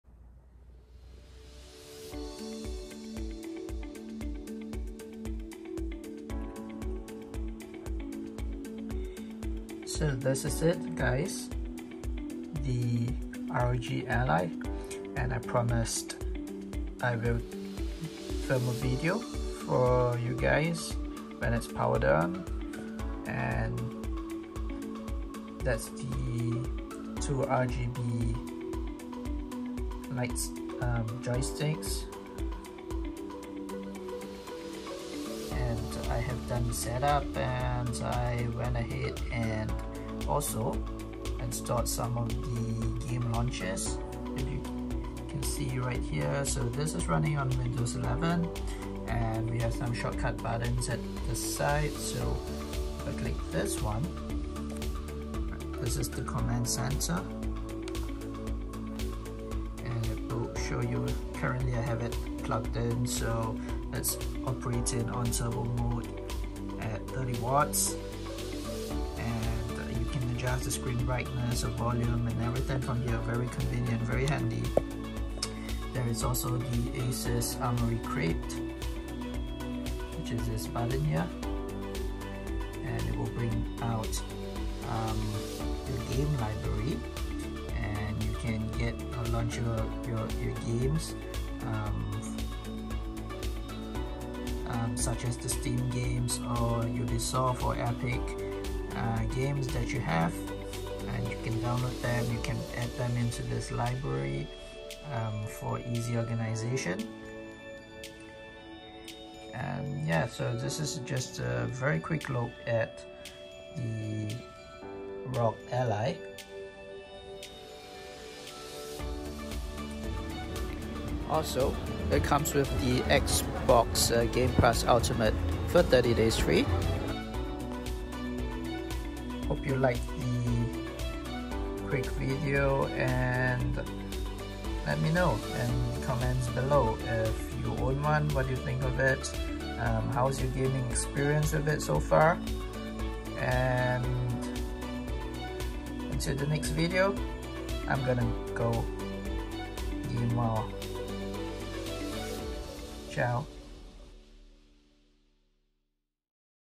Checking out the ROG Alky after updating for the 1st time! Quick run-through
Sharing a basic run through of some of the softwares included with the Asus ROG Ally.